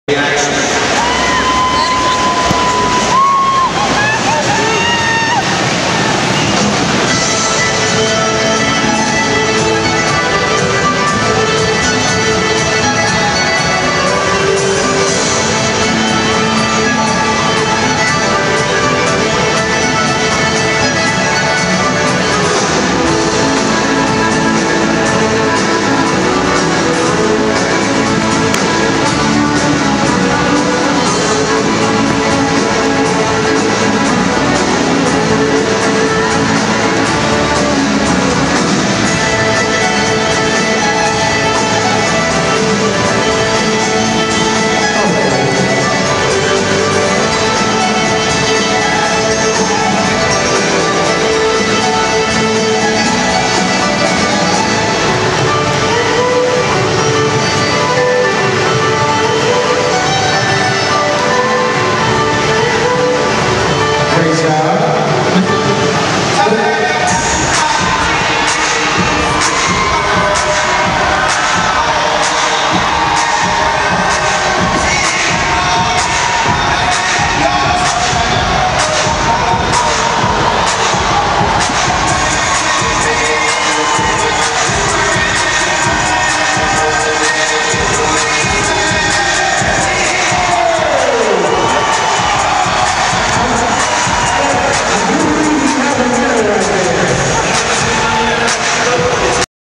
Stefan Performs at GA Force Pregame TCA
Stefan performs at GA Force Arena Football pregame with Force mascot Blu in honor of St. Patricks Day